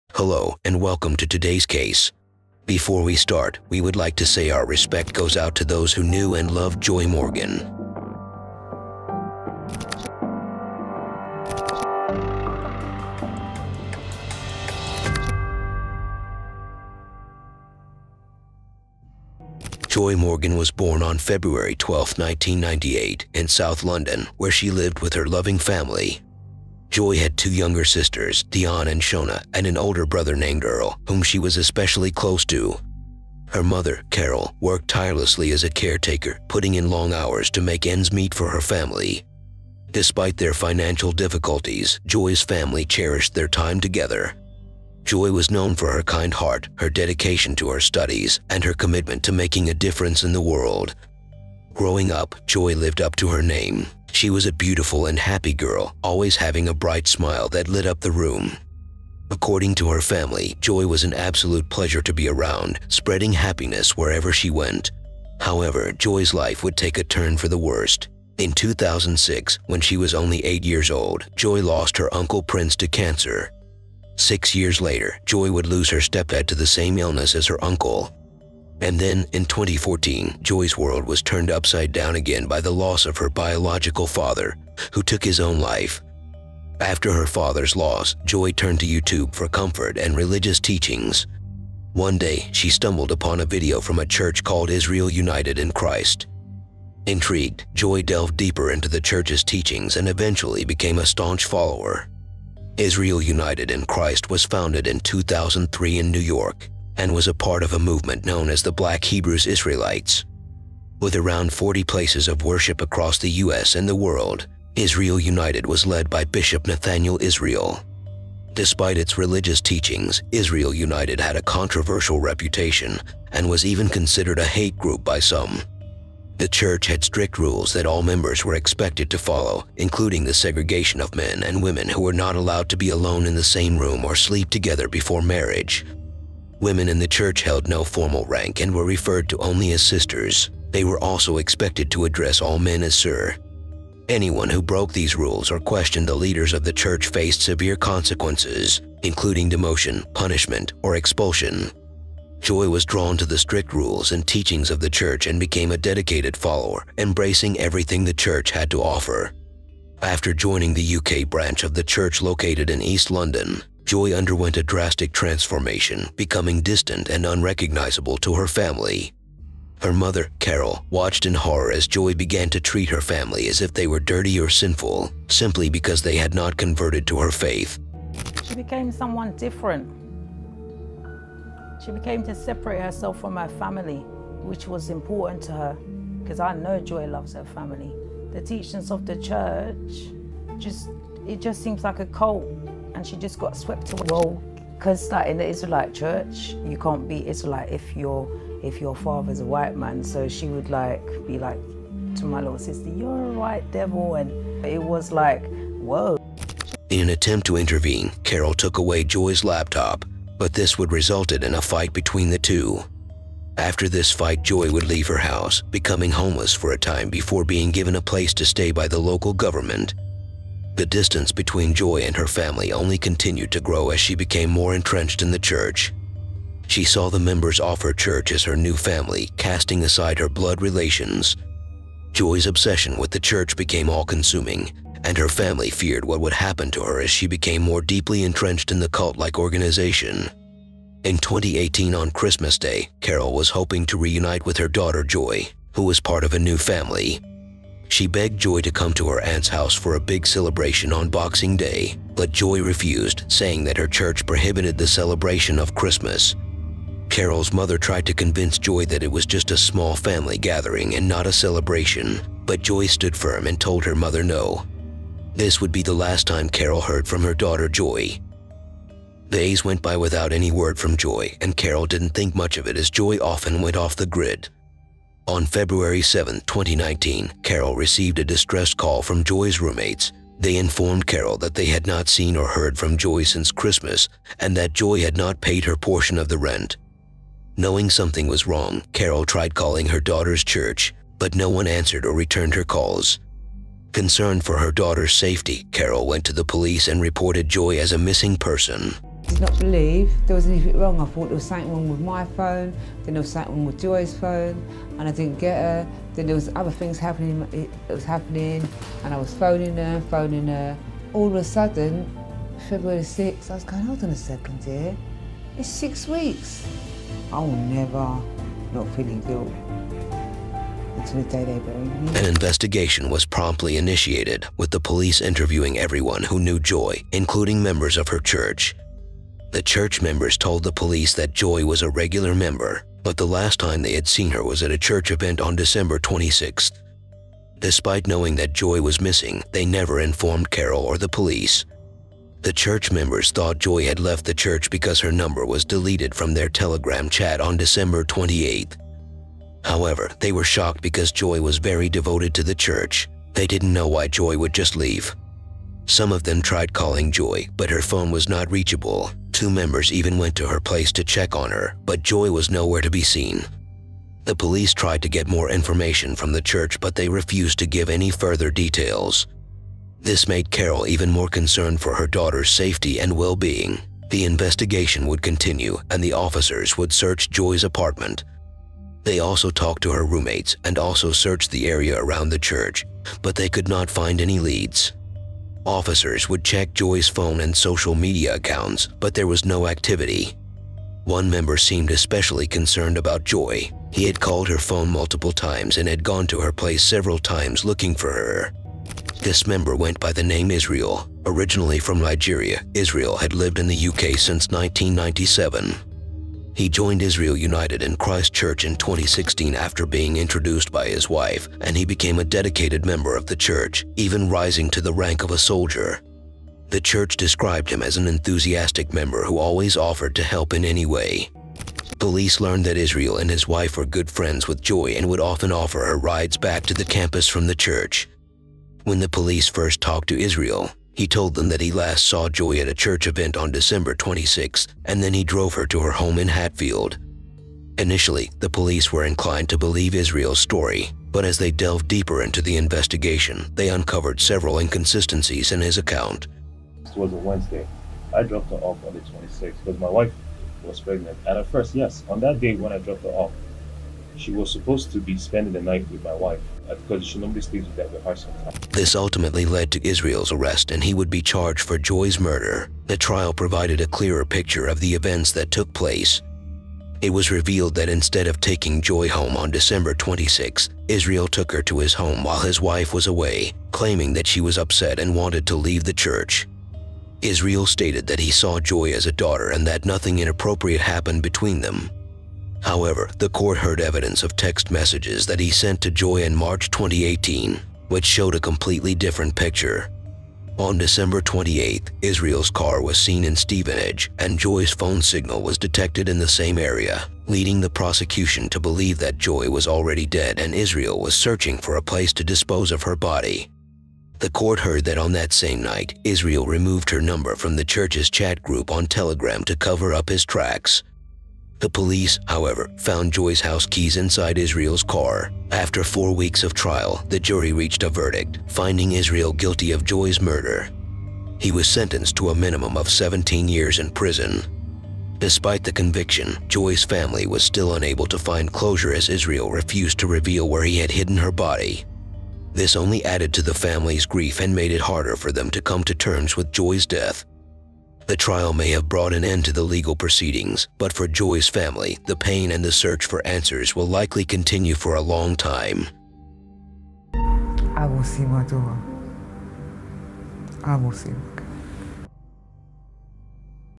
She Was Dr*gged & Killed By A Church Friend | The Disturbing Case of Joy Morgan
✅  - In today's episode, Join us as we take a closer look at the tragic case of Joy Morgan, a student midwife from Hatfield, England who went missing in December 2018. Tragically, her body was later found and a post-mortem examination determined that she had been murdered. In this video, we explore the events leading up to Joy's disappearance, the extensive search efforts to find her, and the eventual arrest and conviction of Shohfah-El Israel for her murder. Through interviews with those close to Joy and expert analysis, we gain a deeper understanding of the events surrounding this heartbreaking case and pay tribute to Joy's memory.